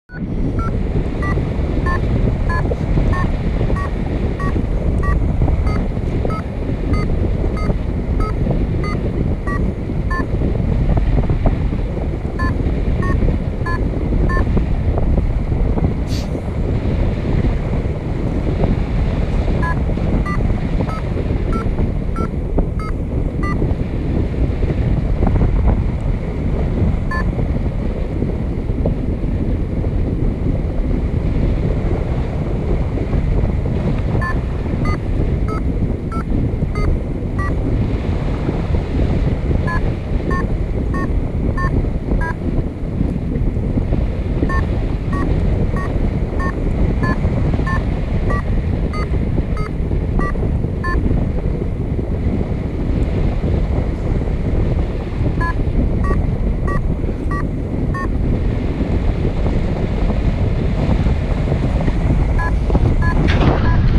190724 Paragliding Chamonix - Plan Praz - Mont Blanc France
Thermalling above Chamonix, opposite Mont Blanc.